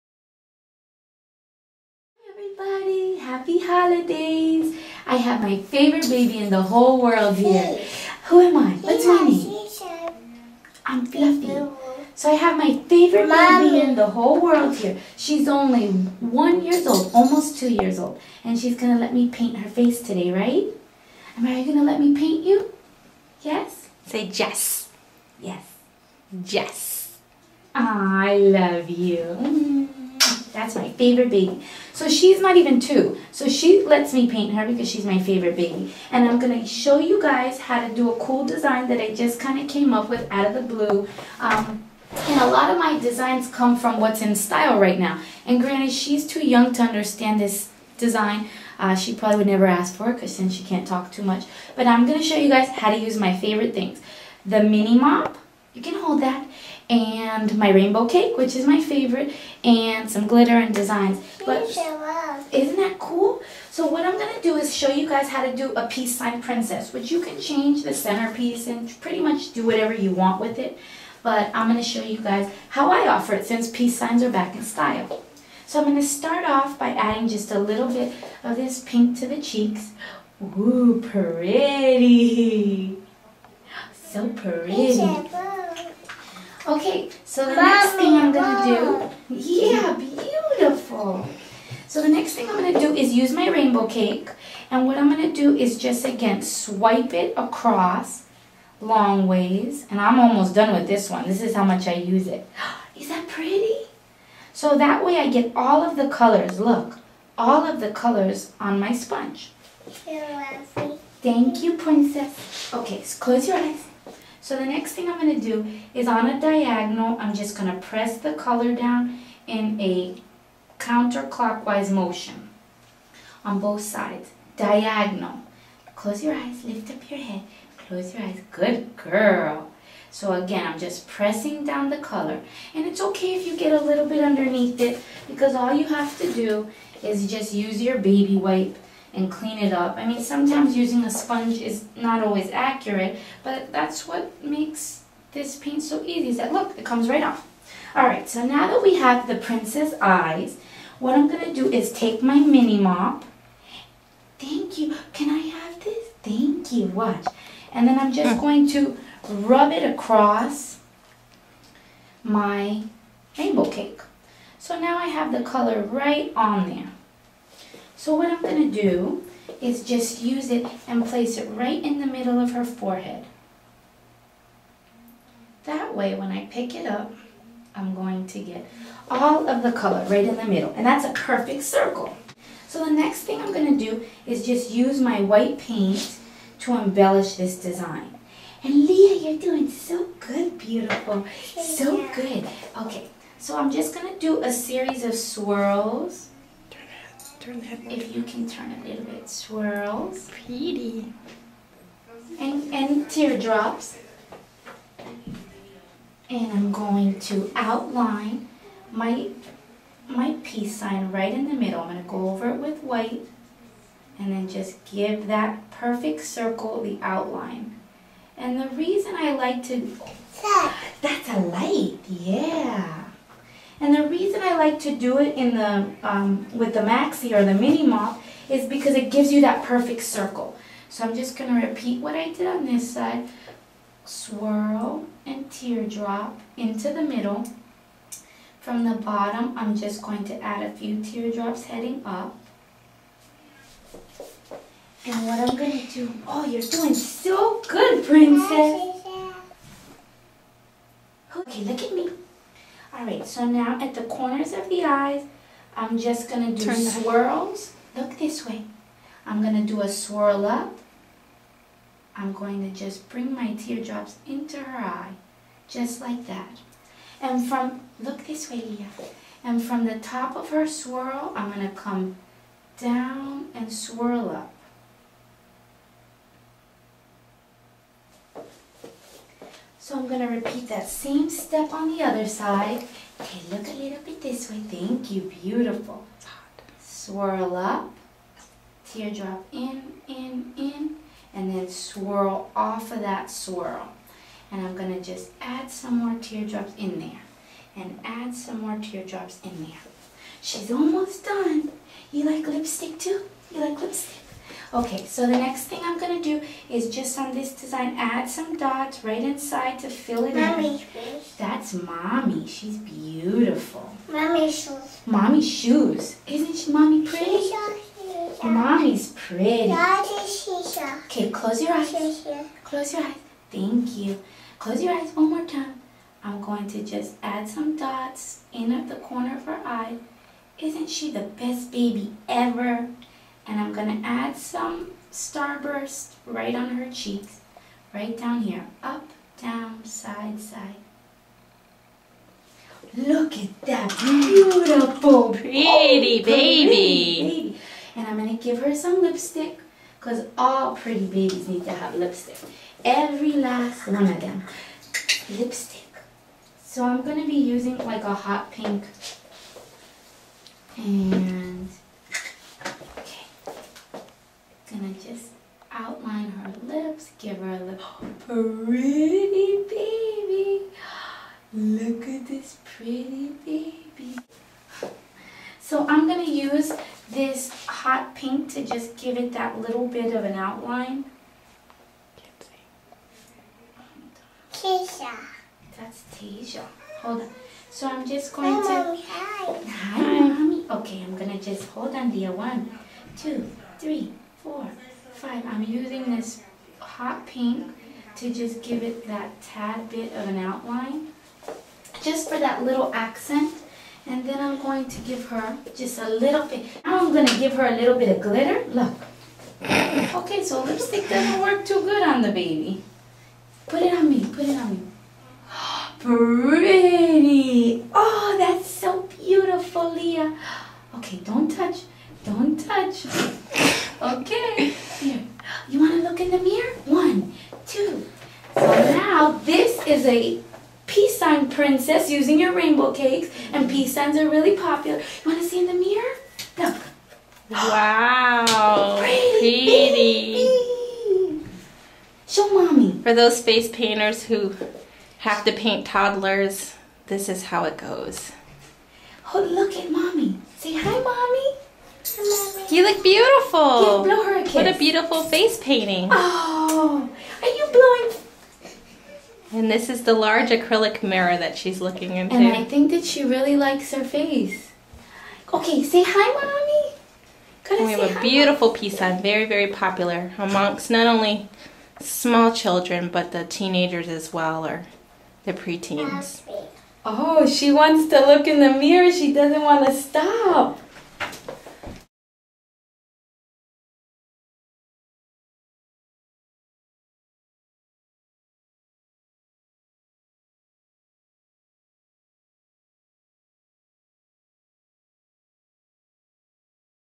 Peace Sign Princess
My little princess Lia models a peace sign rainbow princess as I show how to use a rainbow cake and mini mop